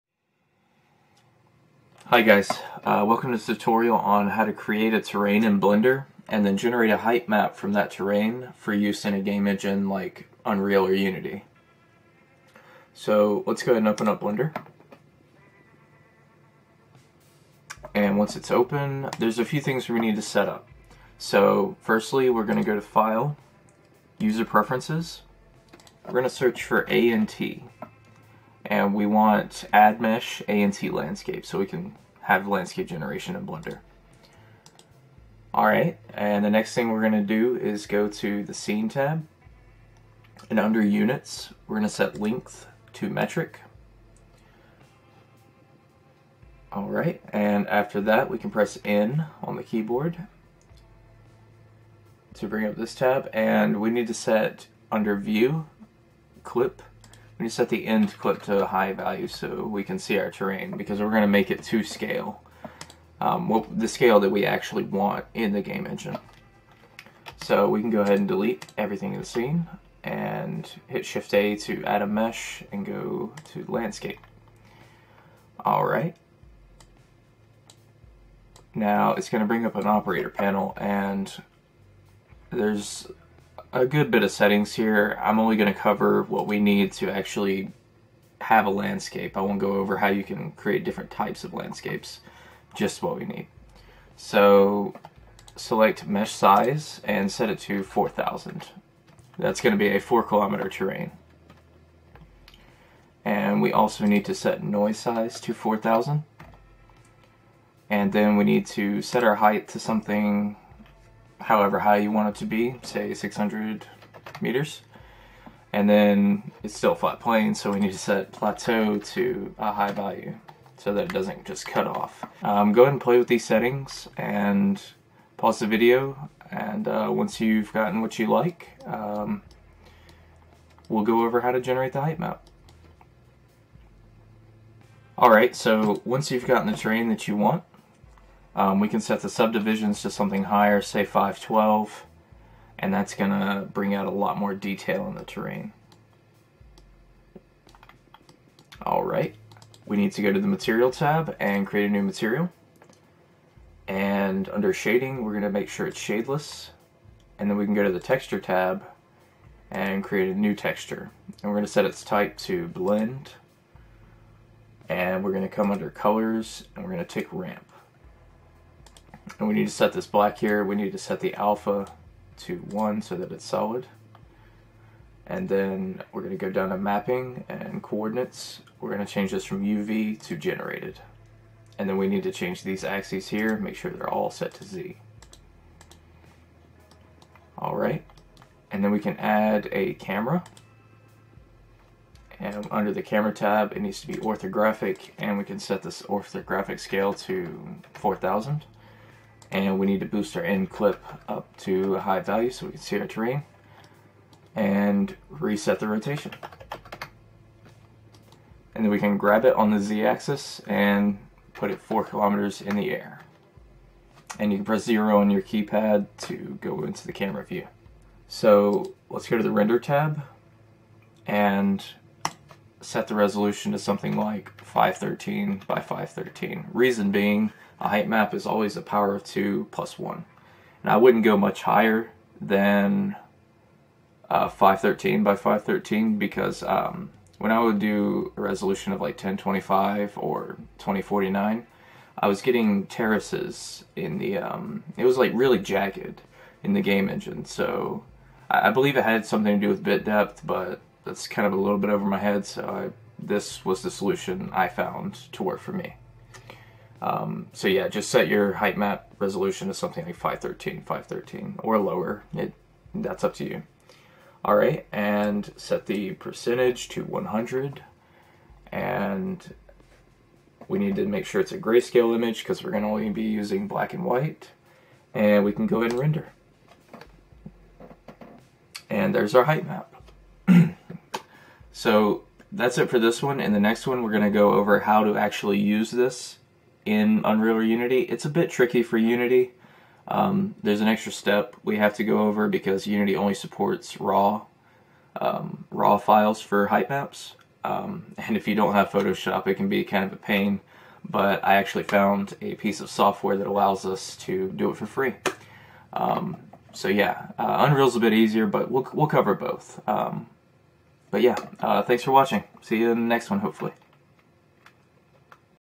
Blender Tutorial: Heightmap from ANT Landscape
In this tutorial we will go over generating a landscape in blender, and then creating a heightmap from that landscape for use in a game engine.